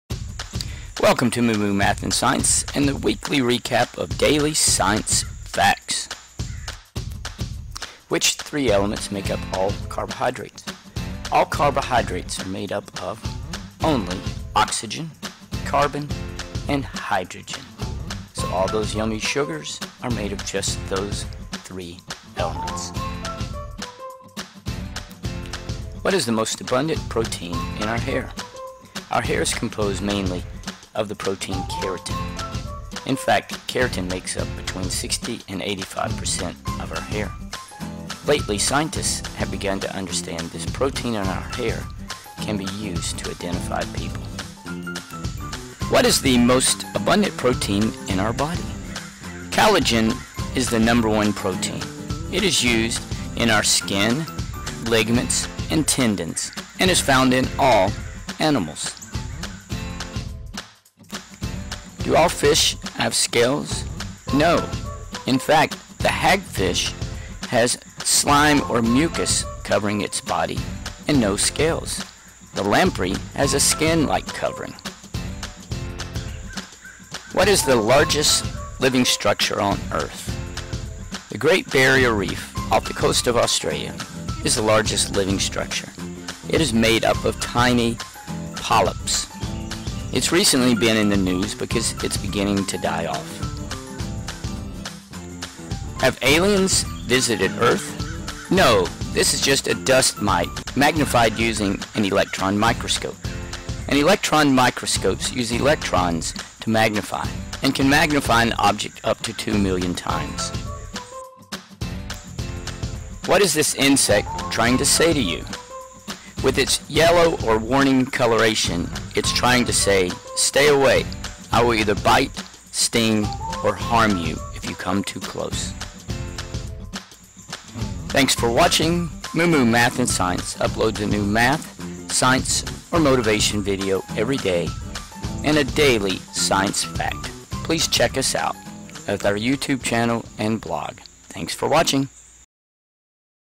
Interesting Science Facts
I hope you enjoy these interesting Science Facts.
Which three elements make up all carbohydrates?
All carbohydrates are made of oxygen,carbon,and hydrogen.
All those yummy sugars are made up of these three elements.
What is the most abundant protein in our hair?
Our hair is composed mainly of the protein keratin.
In fact keratin makes up 60 to 85 % of our hair.
Lately scientists have begun understand you can use this protein to identify people
What is the most abundant protein in our body?
Collagen is the most abundant protein in our body. It is used in our skin,ligaments, and tendons.
It is found in all animals.
Do all fish have scales. No In fact the hagfish has a slimy mucus covering it's body and no scales.
The lamprey has a skin like covering.
What is the largest living structure in the world?
The great barrier reef off the coast of Austria is the largest living structure. It is composed of small polyps.
It has been in the news lately because it is beginning to die off.
Have aliens visited earth?
No this is just a picture of a dust mite magnified using an electron microscope.
An electron microscope uses electronsto magnify, and can magnify up to 2 million times.
What is this insect trying to say to you
It is saying, " with it's yellow or warning coloration it is saying to stay way or I will bite,sting, or harm you if you come to close."